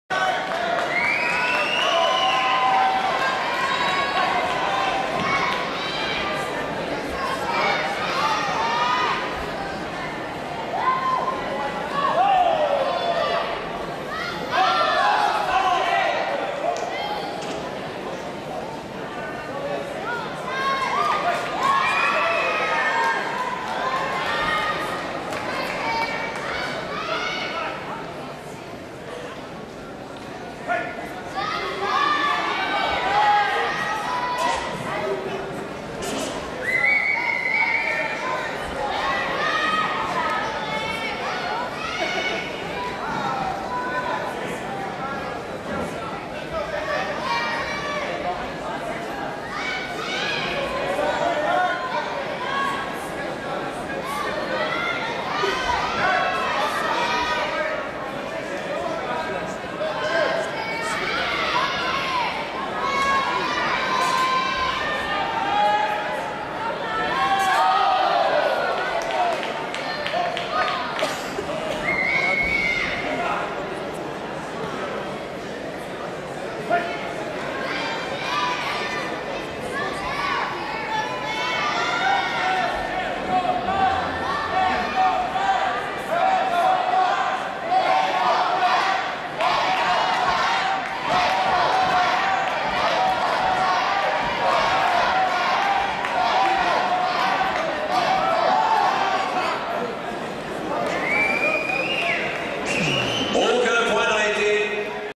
Team Full Circle Canada vs Team Kiraly Round 1 International Team Challenge at Quebec Open 2012
Don't forget to register for the 2012 Gator Nationals May 18-19, 2012.